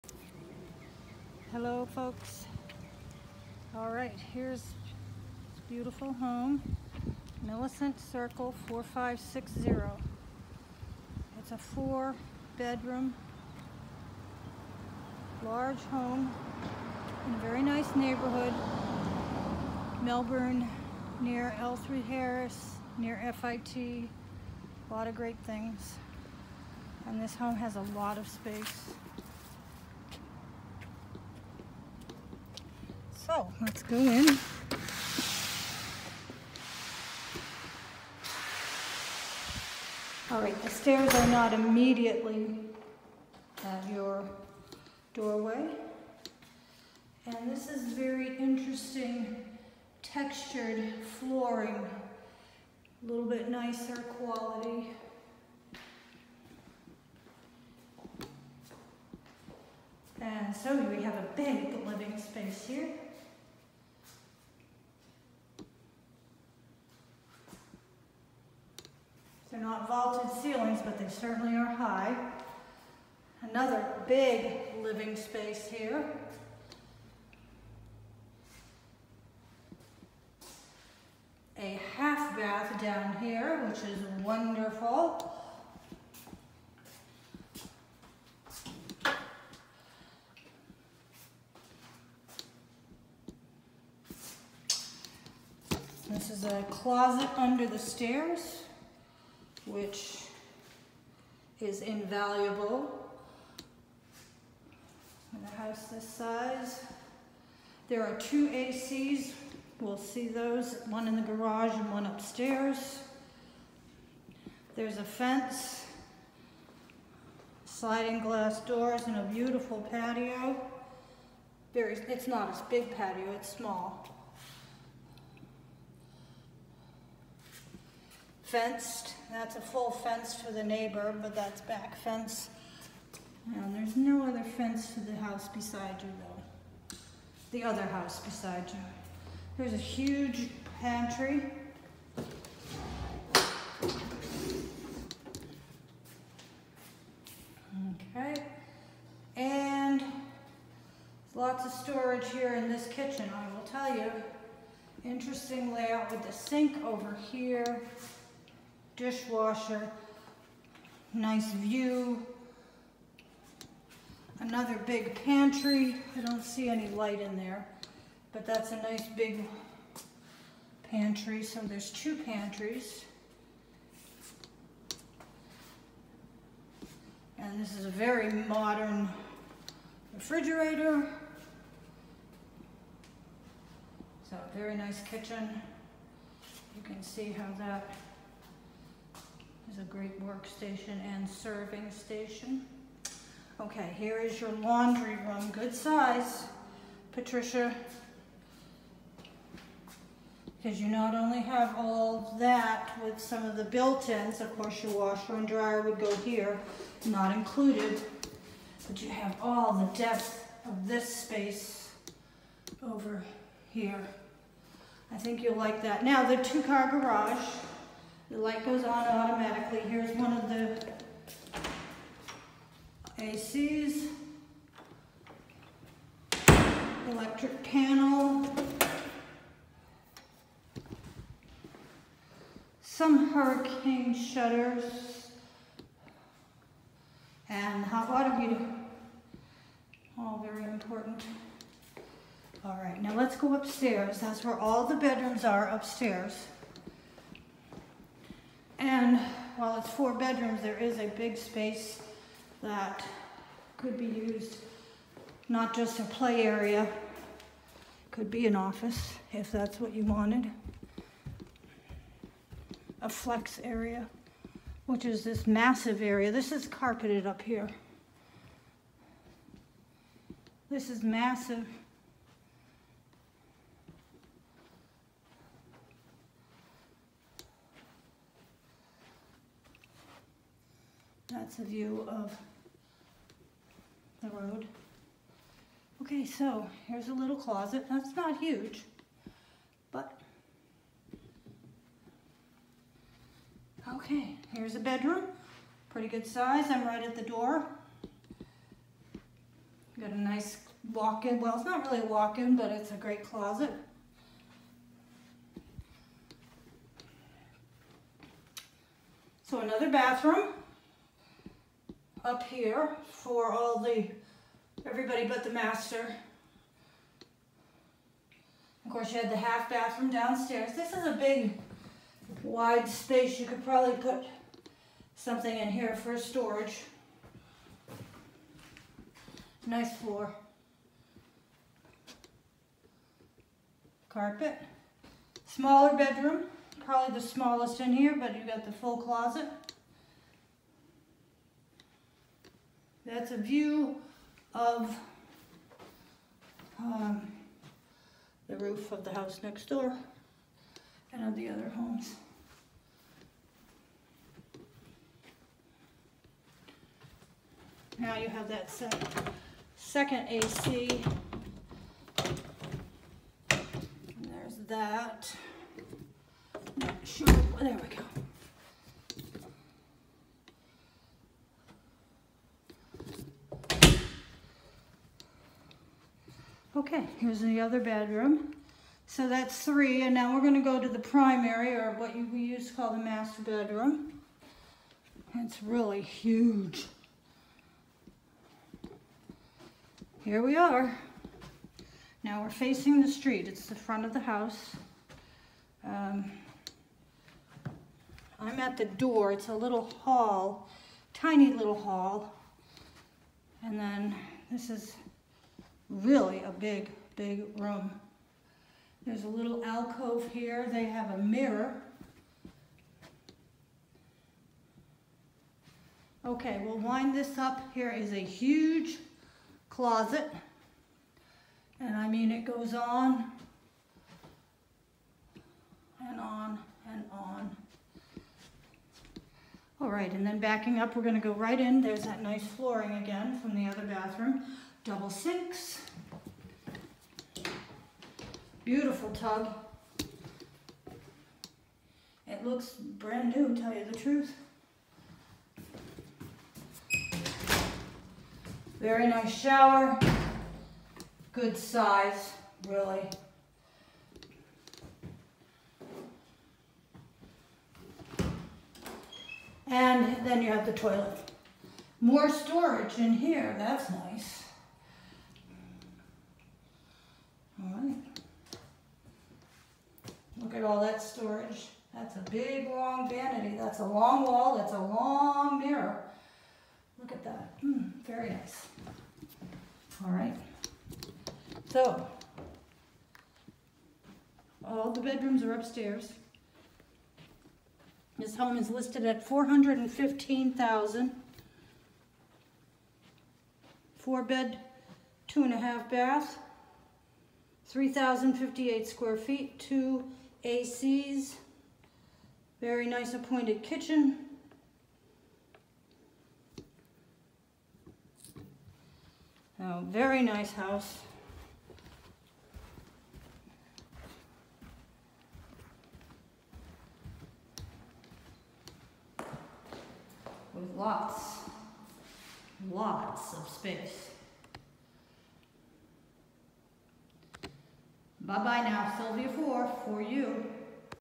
Melbourne 4/2.5 at $415,000.00
4560 Millicent St Melbourne FL.  2008 build, city water & sewer, 3,058 sq ft, new roof, one of two AC's new, HOA $190.00 annually, Taxes: $5,944.11 (non homestead)